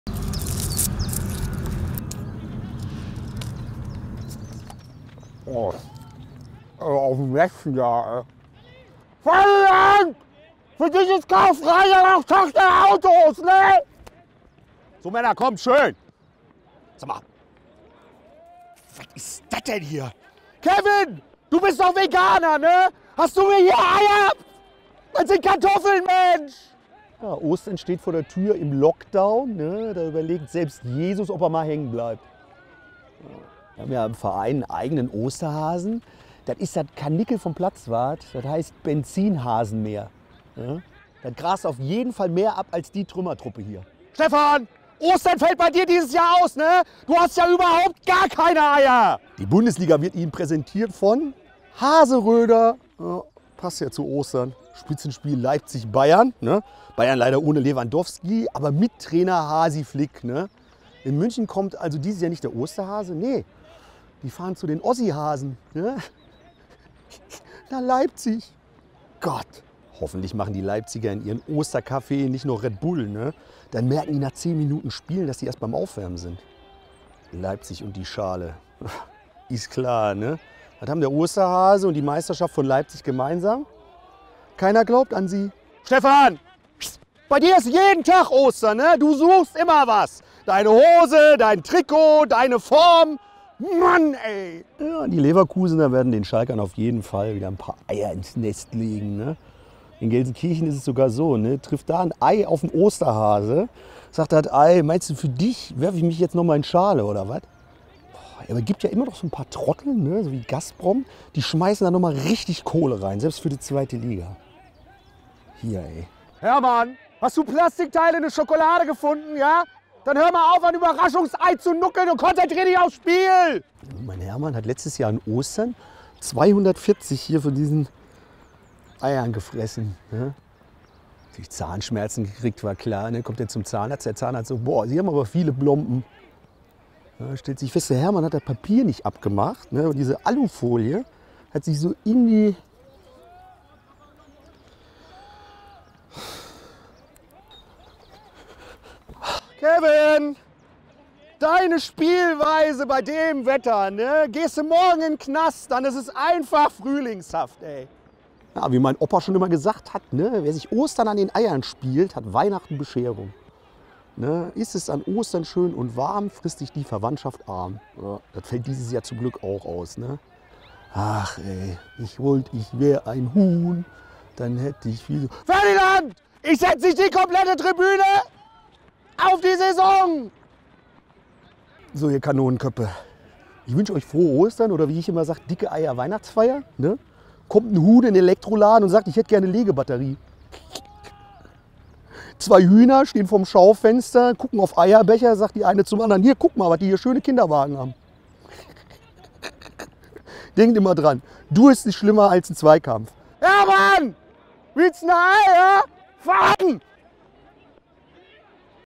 Frohe Ostern & Red Bull Leipzig gegen Bayern München
So ihr Kanonenköppe: Frohe Ostern wünscht euch der UWE!

Lasst es zu Hause ein bisschen gut gehen, schön Bundesliga schauen und ganz wichtiger Tipp:

Von den Schoko-Eiern die Alufolie abmachen